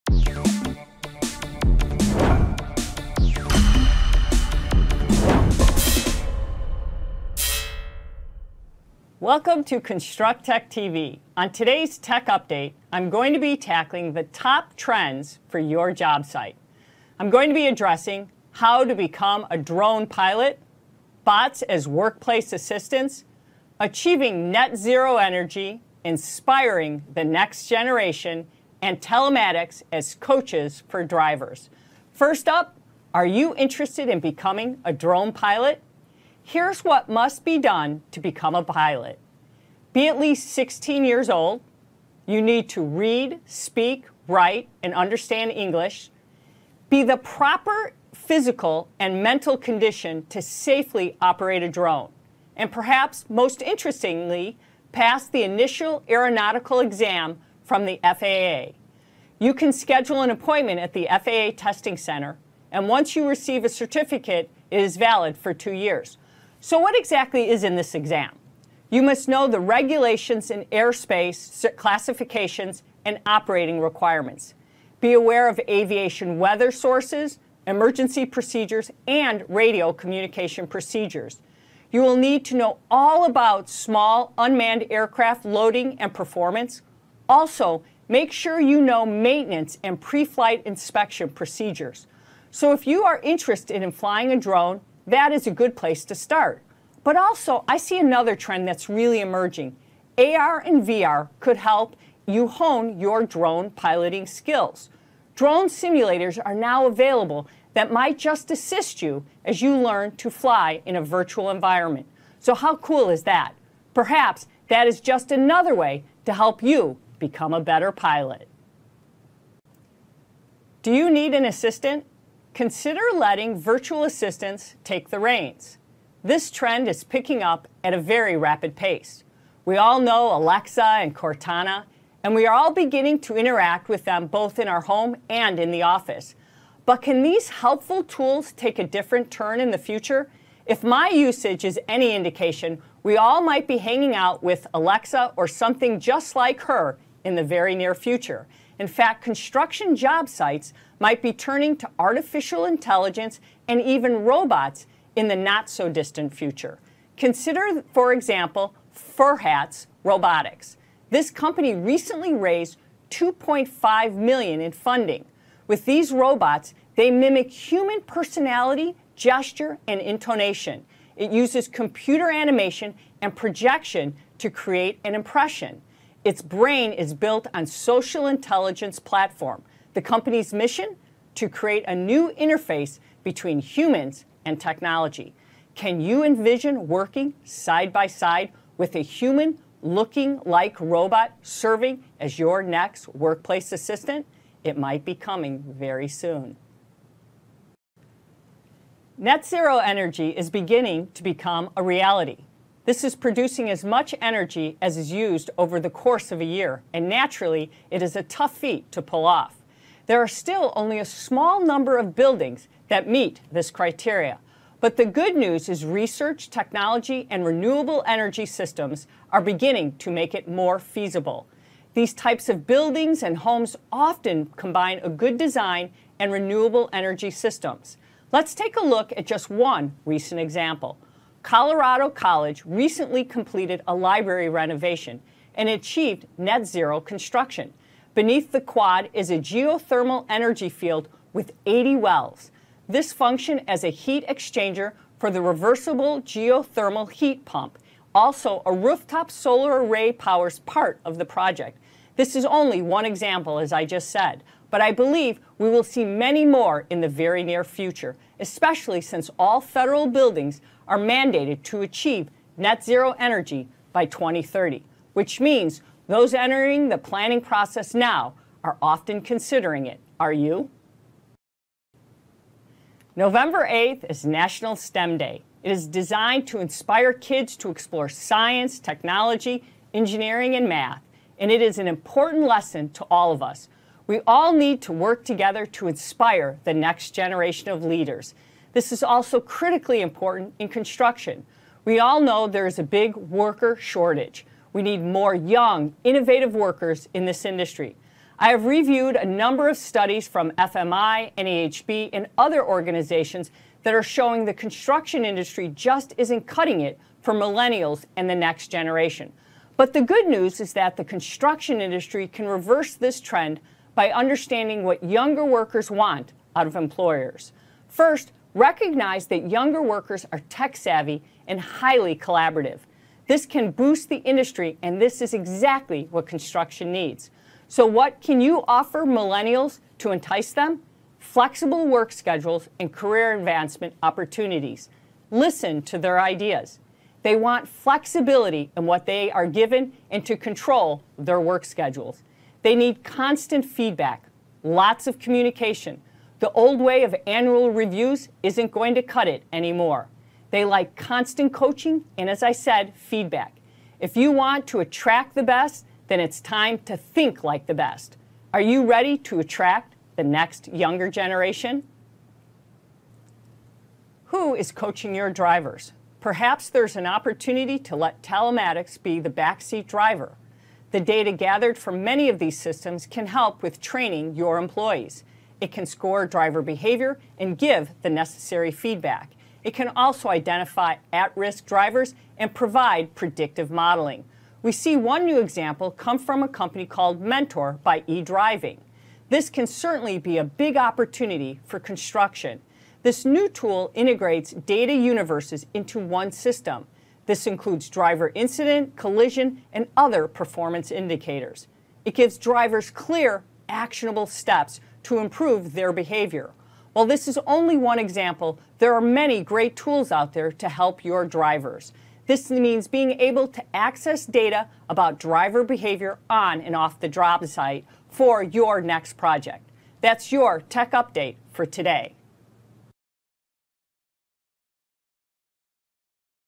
Tech Update, Driving Drones
Peggy Smedley identifies how to become a drone pilot, achieve net-zero energy, and inspire the next generation. She also looks at trends related to bots as workplace assistants and telematics as coaches for drivers.